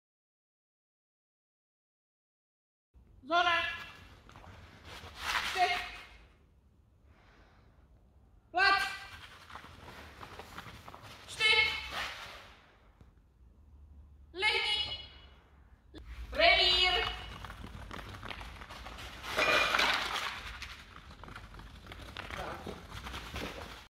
Box vs revir
Někdy je situace na place složitá a je potřeba, aby se pes v těch všech povelech a "kuželech" vyznal. Ráda stavím těžké tréninkové situace, protože pak nás na závodech nic nepřekvapí. Tady byla situace stížená kuželem i boxem, ale i faktem, že jsem kolem kuželu neposílala od nohy ale po vysílačce do kruhu, kde by pes běžně očekával box a dostal revír.
I like to build difficult traning rings as it is important for me that dog is listening to commands and is able to find his exercise/cone within others.